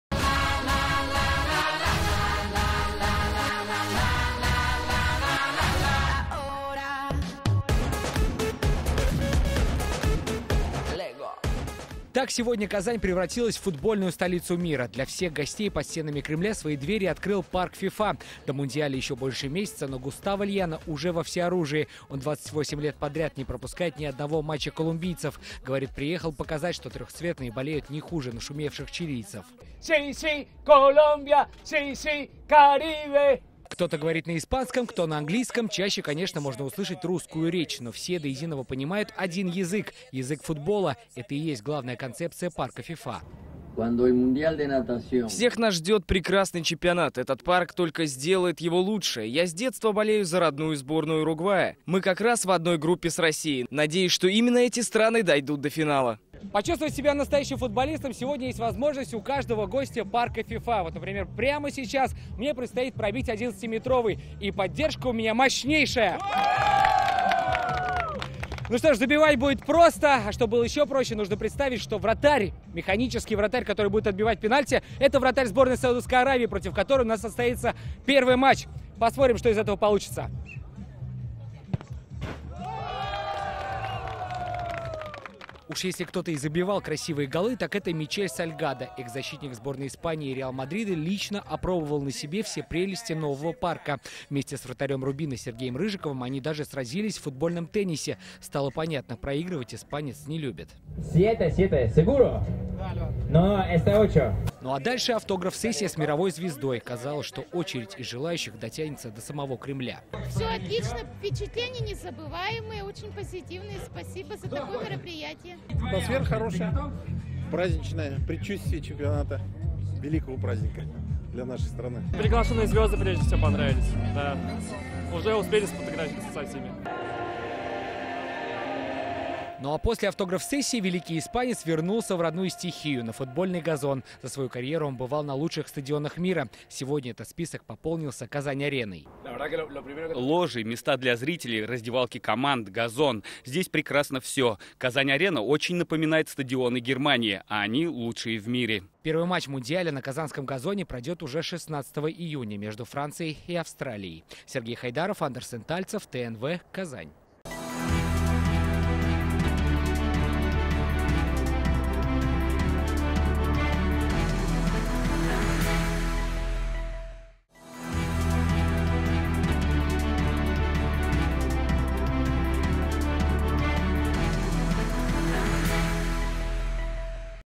В Казани официально открылся парк ФИФА - ТНВ
Познакомились с самым известным болельщиком из Колумбии, и сыграли в футбольный теннис со звёздами мирового футбола. Сегодня в Казани официально открылся парк ФИФА. Сотни зрителей не только оценили спортивный парк, но и пообщались с легендарными футболистами.

Таяну ноктасы. В эфире: пн, вт, ср, чт - 19:00

7 дней. В эфире: вс - 19:30, 21:30

Координаты:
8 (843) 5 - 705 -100